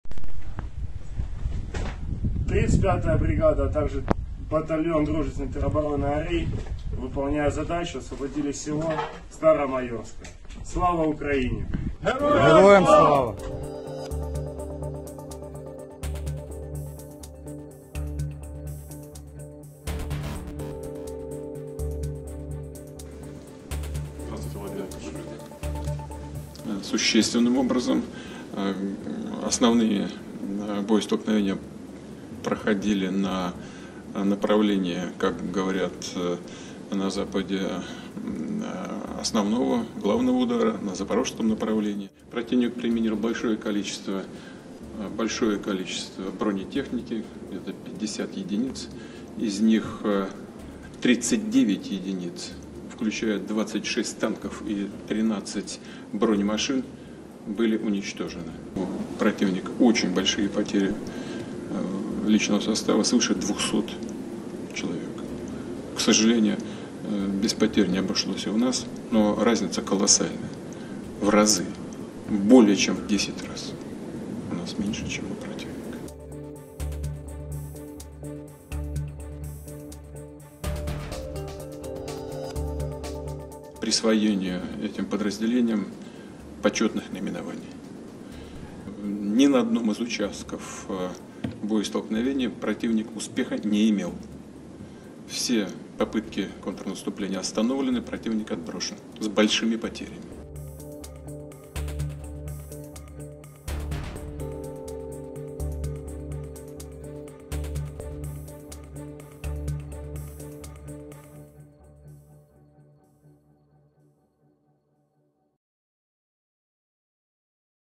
Ukraine claims gains near Zaporizhzhia; Putin admits 'intensified' fights but heavy losses for Kyiv
Ukraine claims it has captured a village of Staromaiorske near the southern front, according to a video released by the office of Ukrainian President Volodymyr Zelensky on Thursday.

The village is 150km (90 miles) east of the city of Zaporizhzhia, where Russian President Putin said “hostilities have intensified significantly”. Speaking to Russian TV, Putin again underscored Kyiv’s unsuccessful “counteroffensive”, saying that Ukrainian forces suffered heavy losses.

Zelensky meanwhile met with military commanders on Thursday for an "offsite meeting" in Dnipro to “discuss counteroffensive situation”.